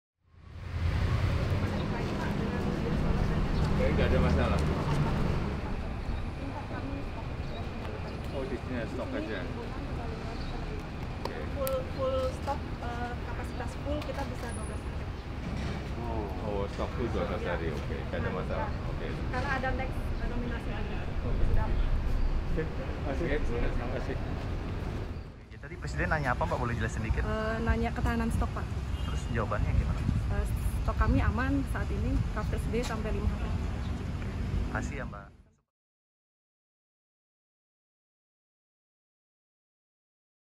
Presiden Jokowi Melakukan Sidak di Terminal BBM Sanggaran, Denpasar, 3 Desember 2021
*Presiden Jokowi Tinjau Terminal BBM di Bali*

Di luar agenda yang sudah dijadwalkan, Presiden Jokowi melakukan peninjauan ke Terminal BBM Sanggaran di Denpasar, Bali, 3 Desember 2021.

Hal itu dilakukannya dalam perjalanan setelah menghadiri acara Peluncuran Peta Jalan Ekonomi Kerthi Bali di Kura-Kura Bali, Kota Denpasar.

Di Terminal BBM milik Pertamina itu, Presiden Jokowi menanyakan ketersediaan stok BBM. "Bagaimana stok BBM?" Ujar Presiden.

Ayu Kurniawati, Supervisor Receiving, Storage and Distribution Region V Pertamina mengatakan kepada Presiden bahwa stok dan pengiriman BBM ke wilayah Bali aman.

Tampak mendampingi Presiden Jokowi Menteri Sekretaris Negara Pratikno.